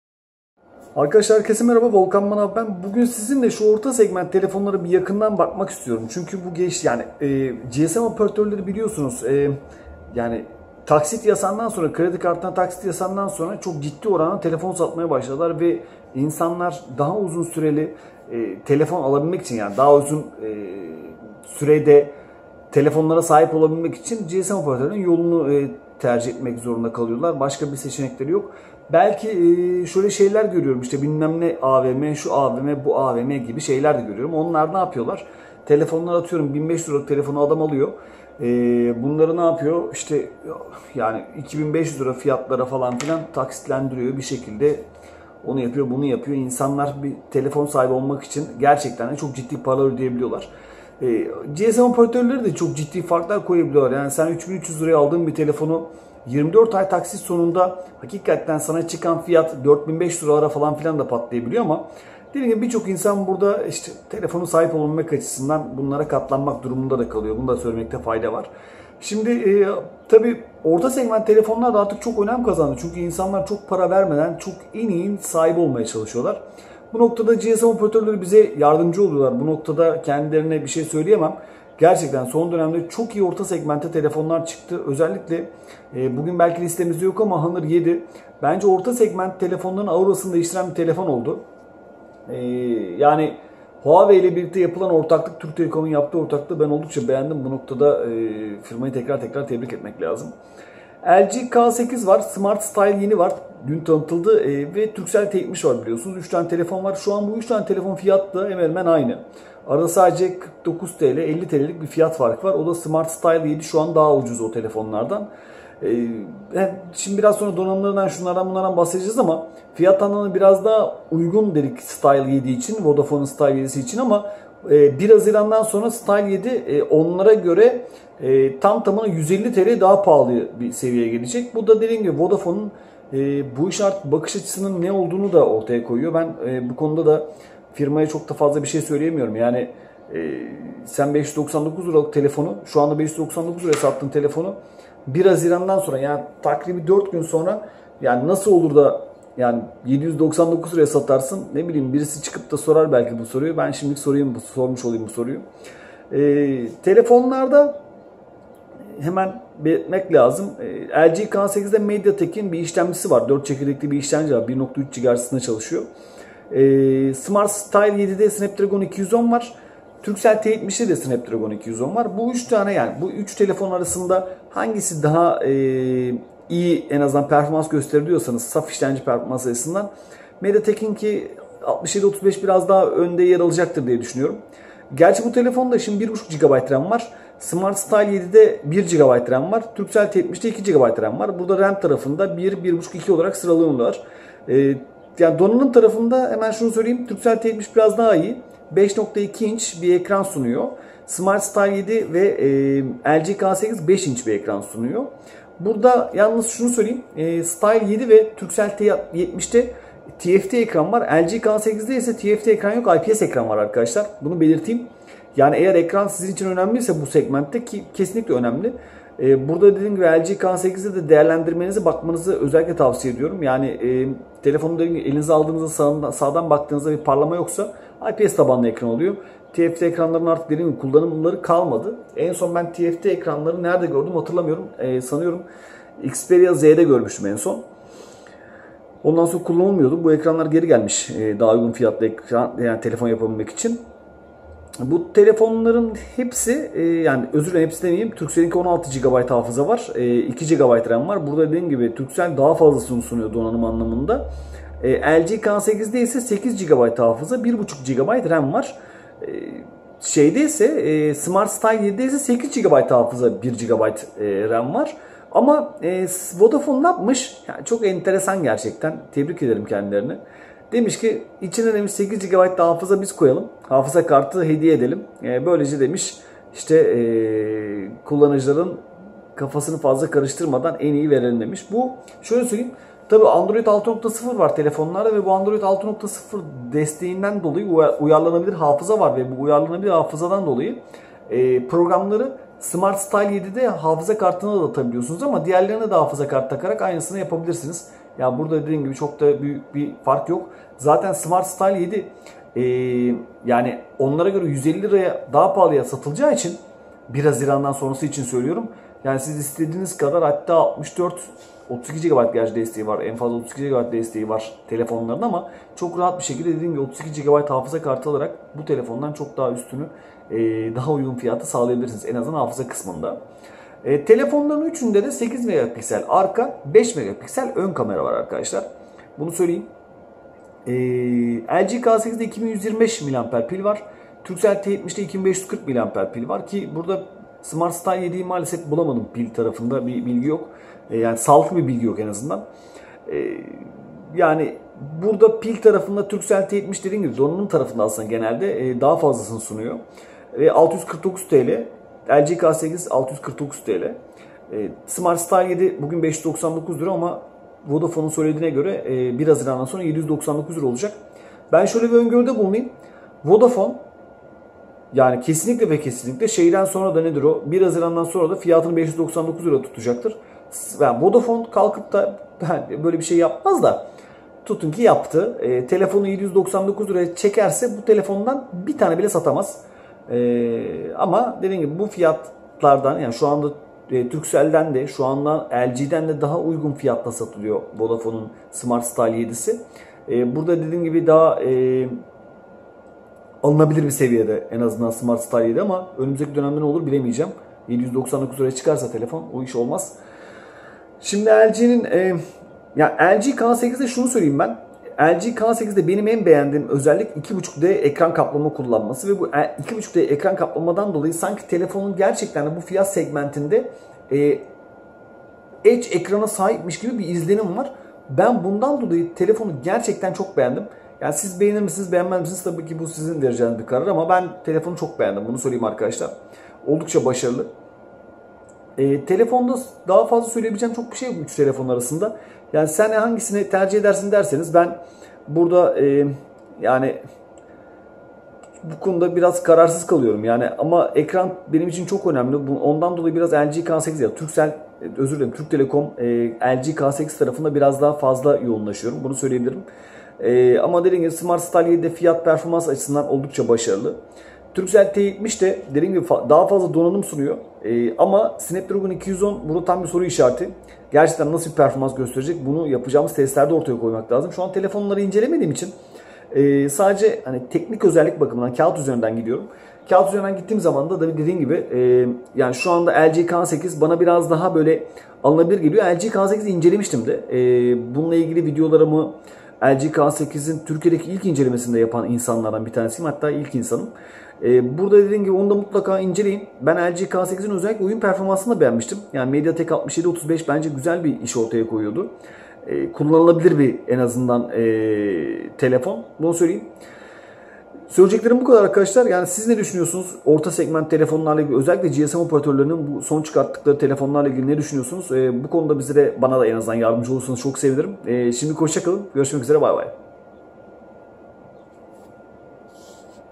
LG K8 mi? Turkcell T70 mi? Vodafone Smart Style 7 mi?
GSM Operatörü telefonlarının ön değerlendirmesi ve tüm özelliklerini değerlendiriyoruz...